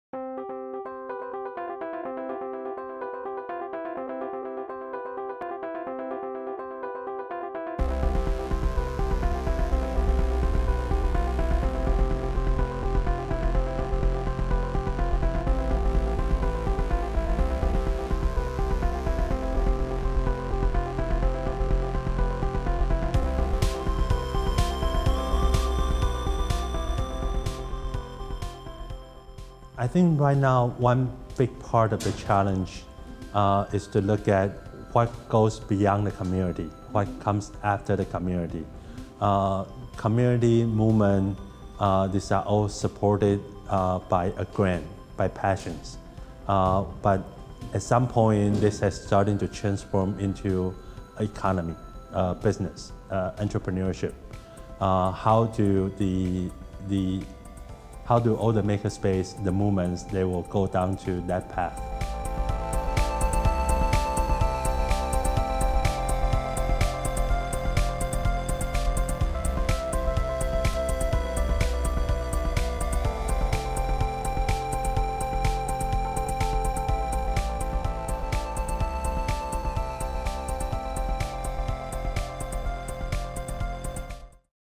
Which are the challenges for Maker CAPS? David Li, Shenzhen Open Innovation Lab
Fab City Experts talked about the challenges that CAPS for Makers will have in the future during the Fab City Summit in Amsterdam, April 2016. Some MAKE-IT partners worked in the Fab City Whitepaper draft in Amsterdam Makerversity and Fab City Campus, and showed some of the conclusions at Park Huis de Zwiger.

Fab City is a global project to develop locally productive and globally connected self-sufficient cities. It comprises an international think tank of civic leaders, makers, urbanists and innovators working on changing the paradigm of the current industrial economy where the city operates on a linear model of importing products and producing waste.

David Li has started several open source software projects and contributed to many others. In 2010, he co-founded XinCheJian, the first hackerspace in China to promote hacker/maker culture and open source hardware. In 2011, he co-founded Hacked Matter, a think tank on makers and open innovation. In 2015, he co-founded Maker Collider, a platform to develop next generation IoT from Maker community. And, as director of Shenzhen Open Innovation Lab, he has been committed to promoting the development of Chinese makers and the transformation of innovative products.